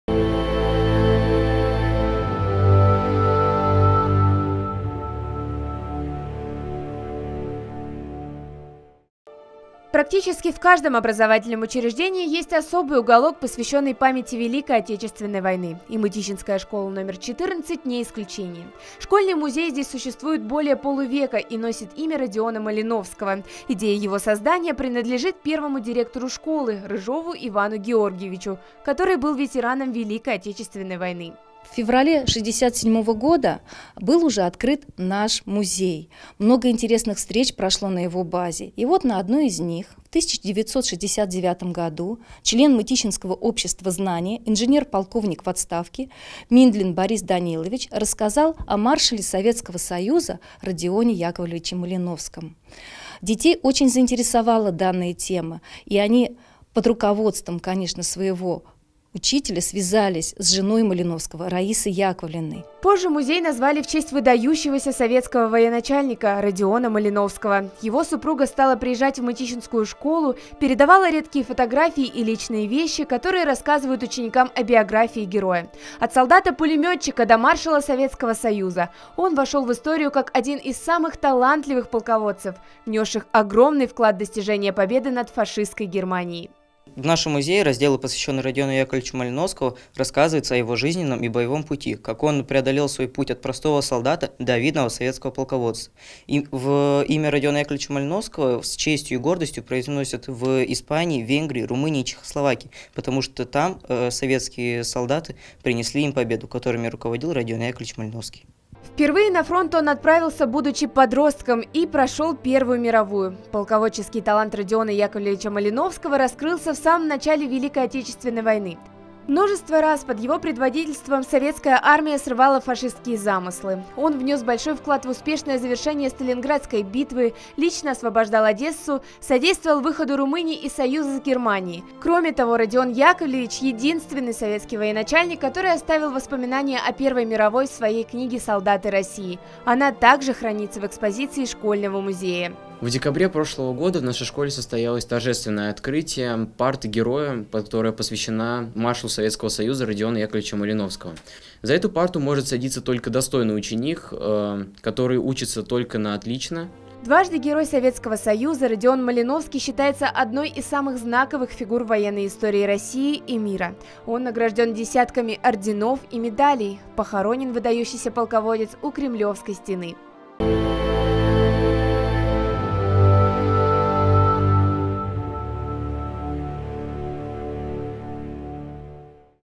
«Вечно-живые»: Маршал Советского Союза Р. Я. Малиновский
Мы продолжаем нашу историческую рубрику «Вечно-живые», в которой рассказываем о людях бессмертного подвига. Сегодня речь пойдет о Советском военачальнике, Маршале Советского союза Родионе Яковлевиче Малиновском. Его имя носит музей Боевой славы в школе №14. Знания о выдающемся полководце учащиеся передают из поколения в поколение. (12+)